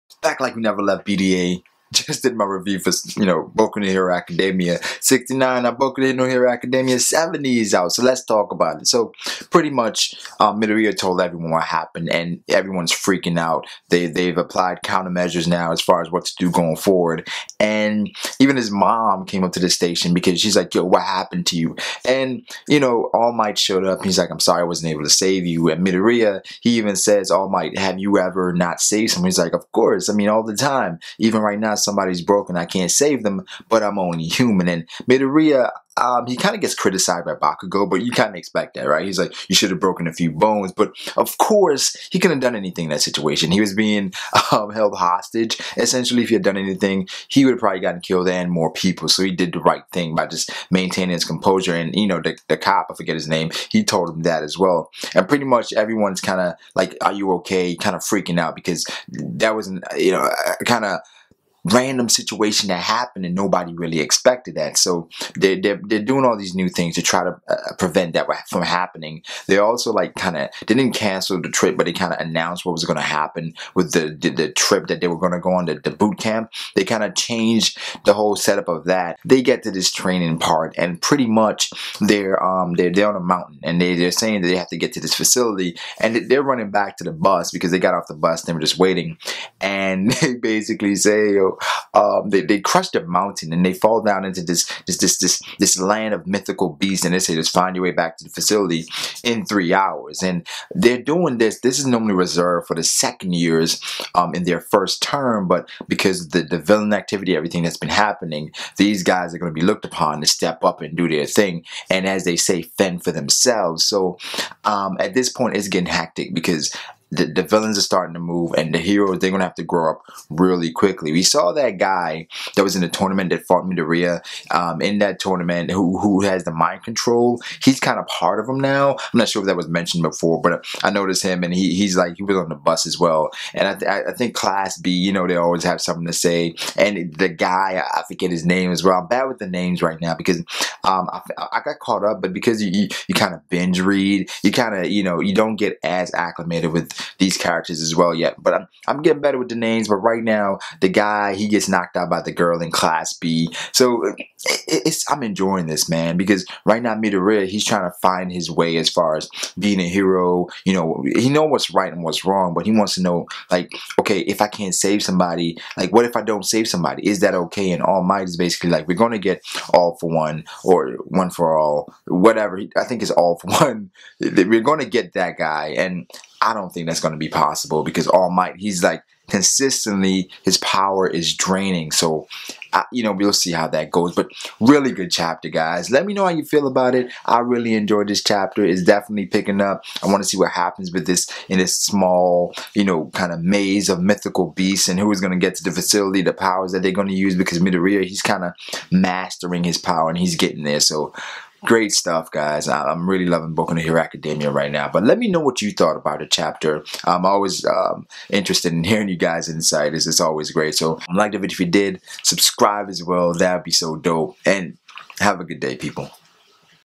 My Hero Academia Chapter 70 Review | Fend For Yourself! -僕のヒーローアカデミア
Villains are on the move and the first years need to step up, boot camp time! More after the click.